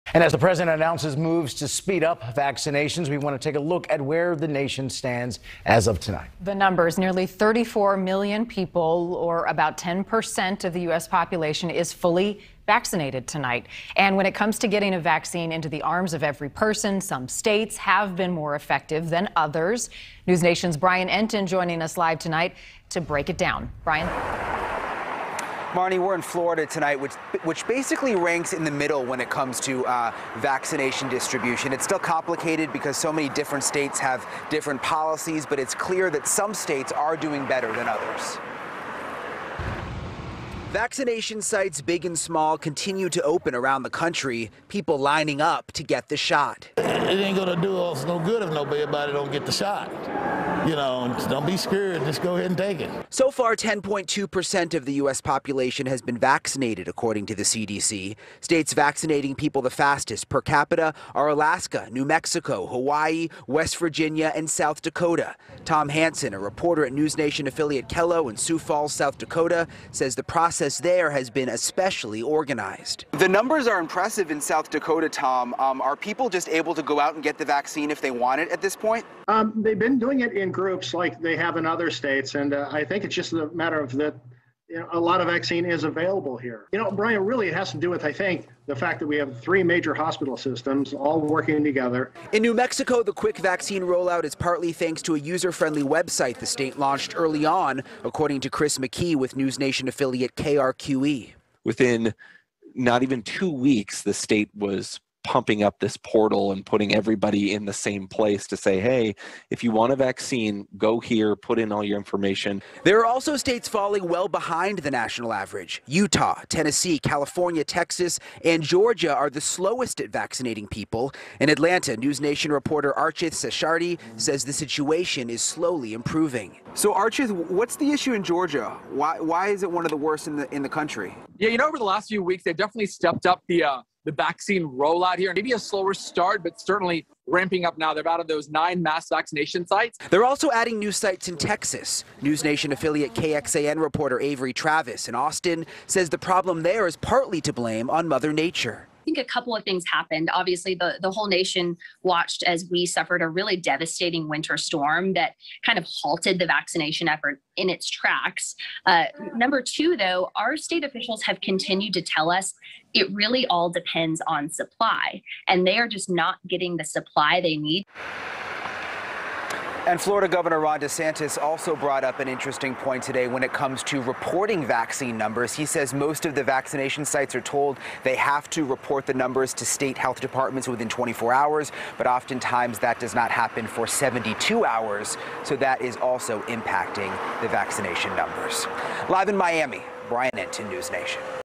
Tracking vaccinations across the nation as more sites open
As President Joe Biden announced moves to speed up vaccinations across the nation Thursday, vaccination sites — big and small — continue to open around the country.